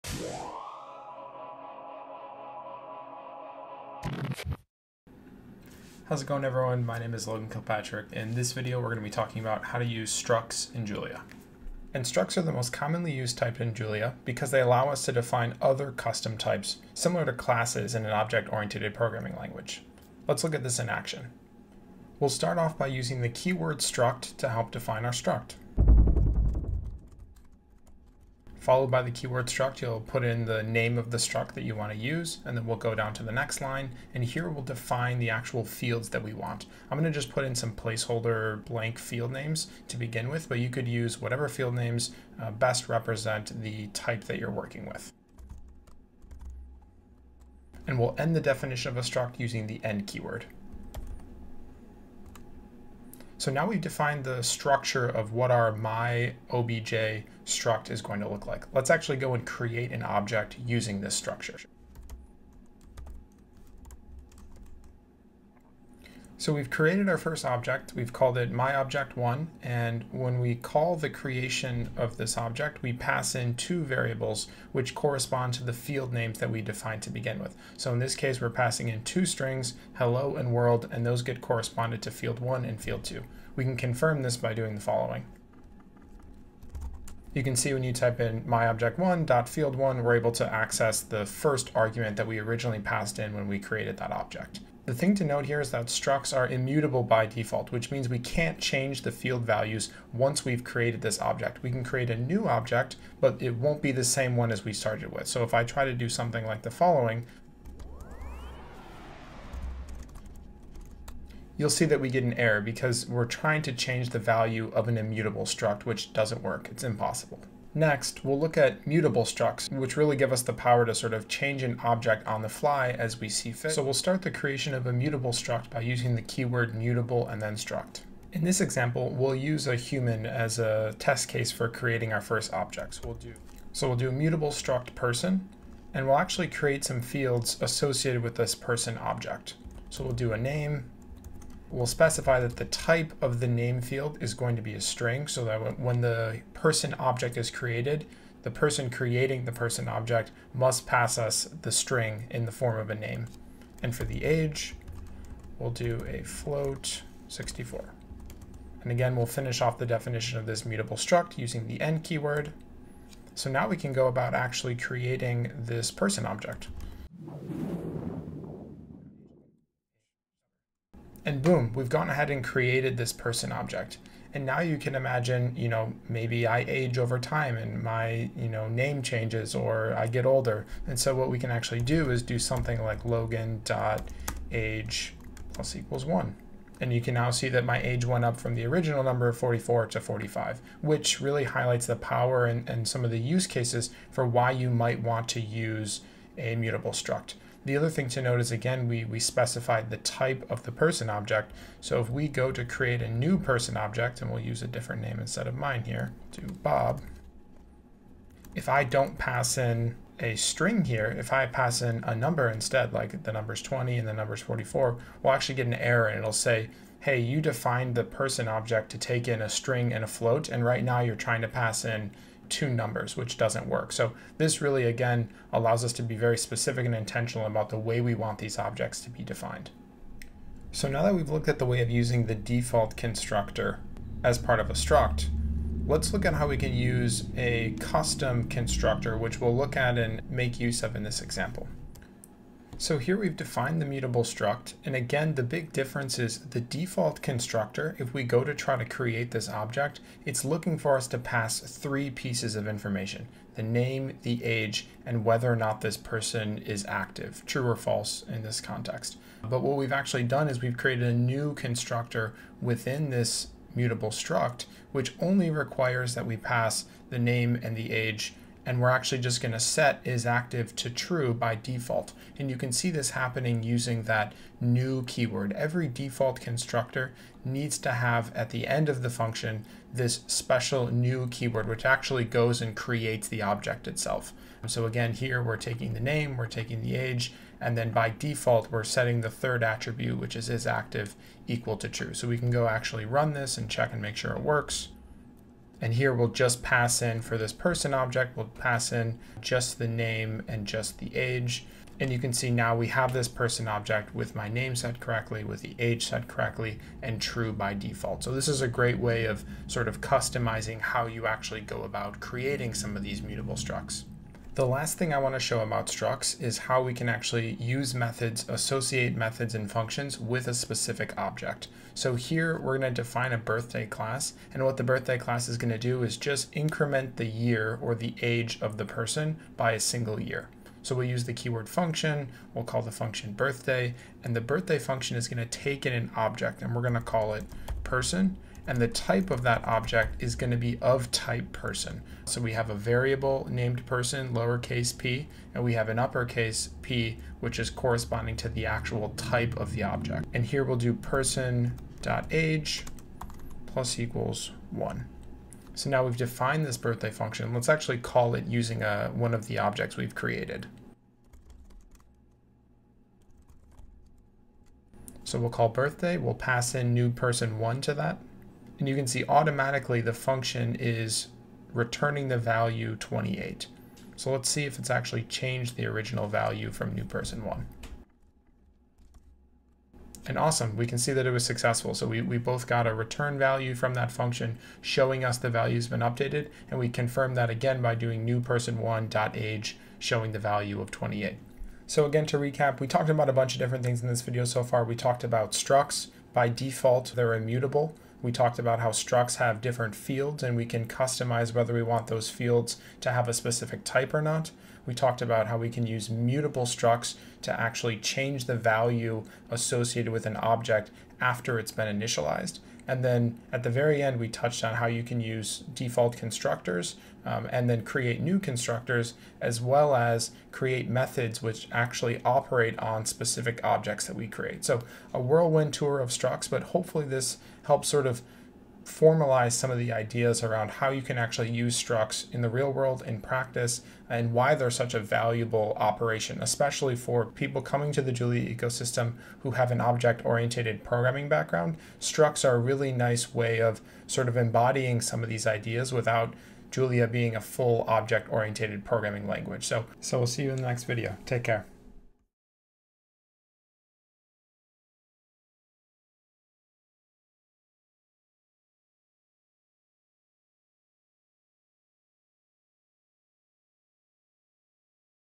Exploring Structs in the Julia programming language with Logan Kilpatrick
In this video, we will explore the basics of Structs in Julia using constructors, fields, types, and methods.

00:00 Welcome!
00:10 Help us add time stamps or captions to this video! See the description for details.